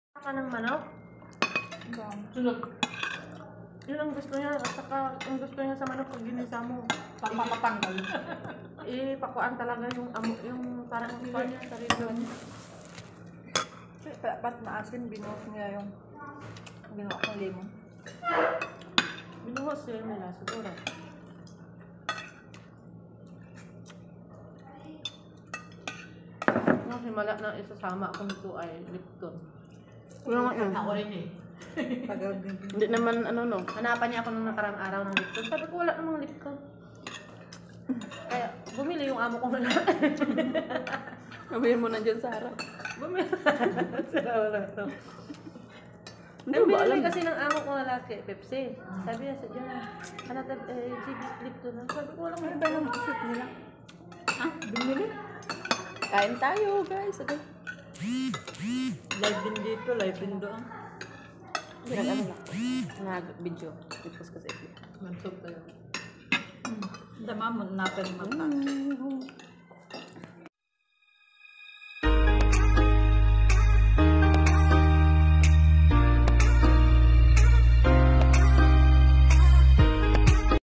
Sarap ng aking kain kasi sinigang na manok yong ulam 😋😋 ||Merzkie Life
Ganito talaga ako kapag kumakain 😀.Sarap ng ulam sinigang po kaya naparami yong kain.